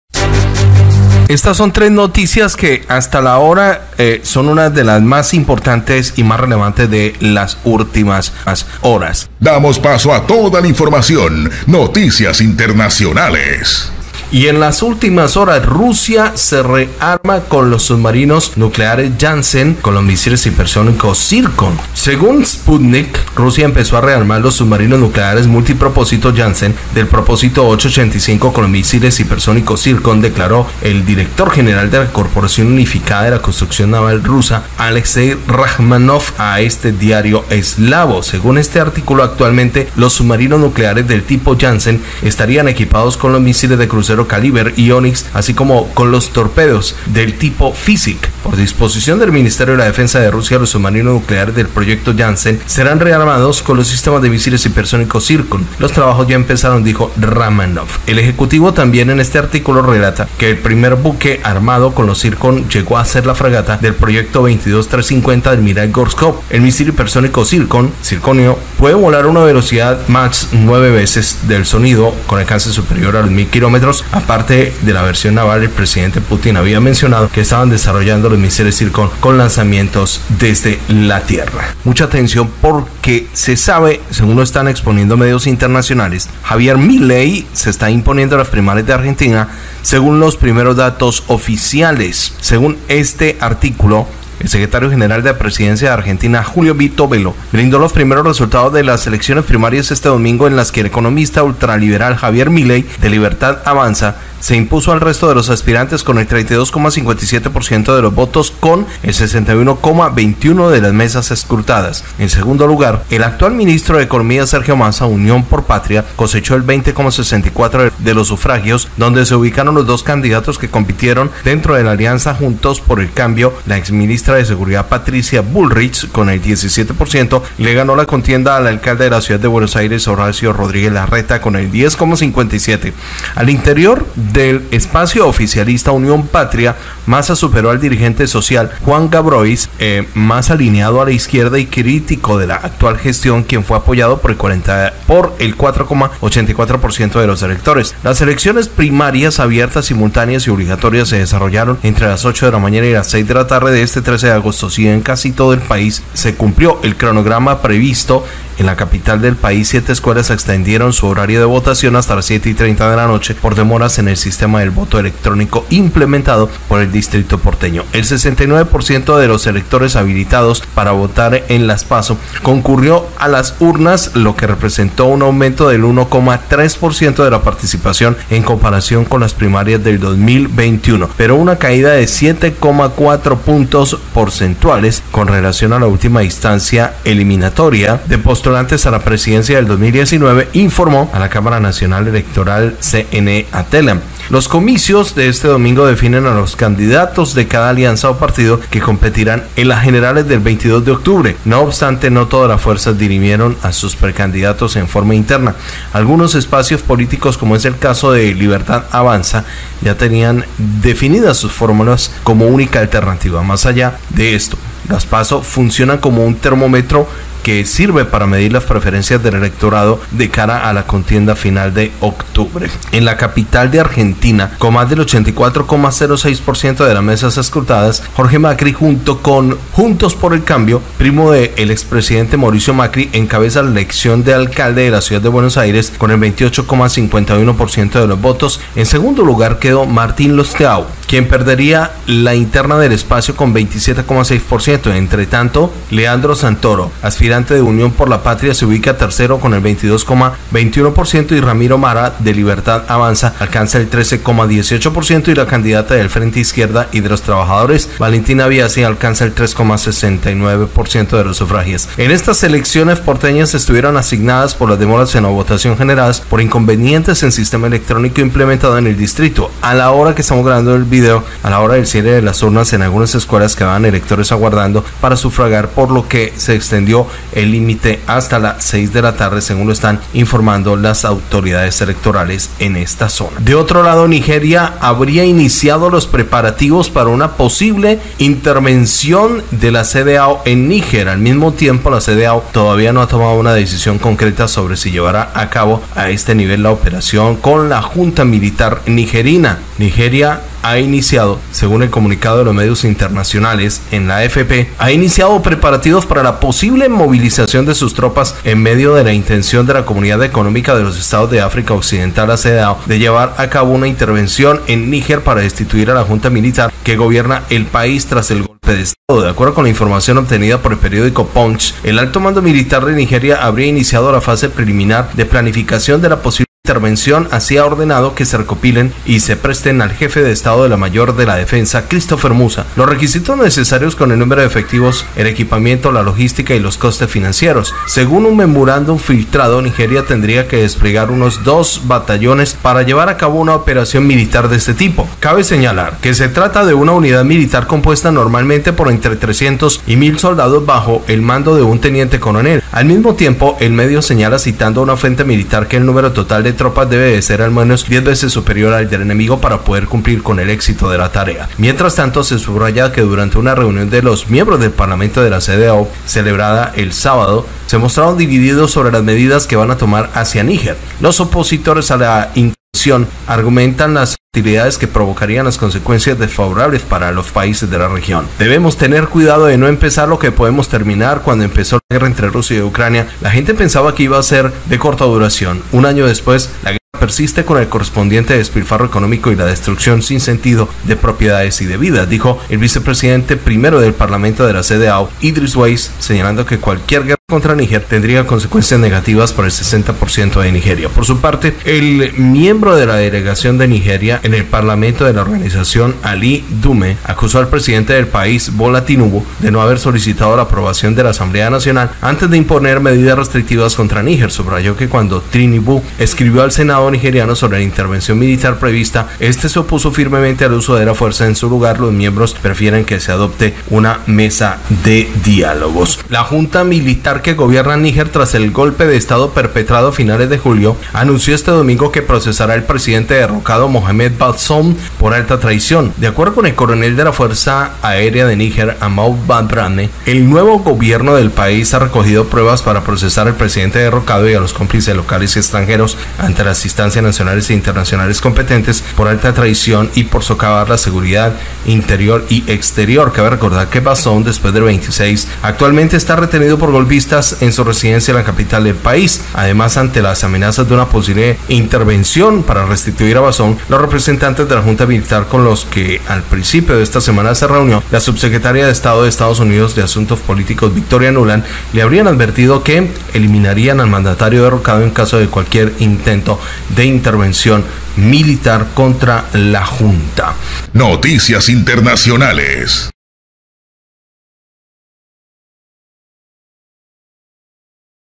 🚨LO ÙLTIMO Rusia Rearma sus Buques con Misiles Zircon PASO en Argentina Nigeria y CEDEAO analizan .
✅Rusia anuncia que rearmas sus Buques de guerra con misiles Hipersònicos.

✅Detalles de las PASO en argetina

✅CEDEAO discute pasos a seguir y una posible intervenciòn en NIGER.